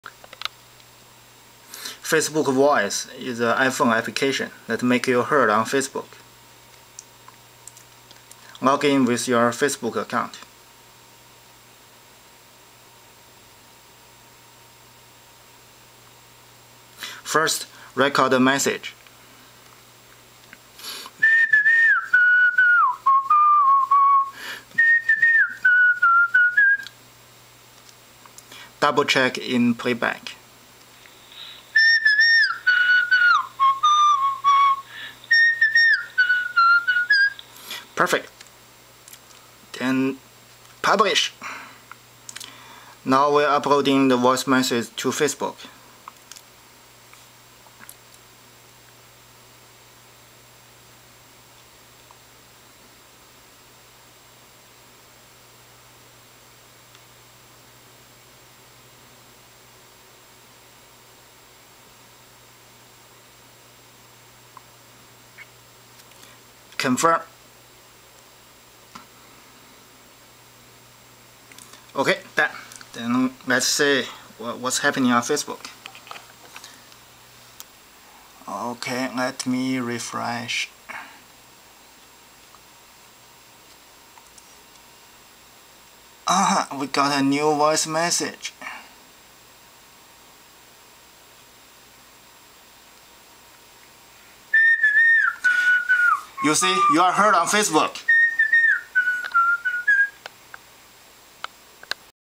How to make yourself heard on facebook
Want to make yourself heard on facebook? Want to share voice message on facebook? FacebookVoice and FbVoicePro can help to achieve it.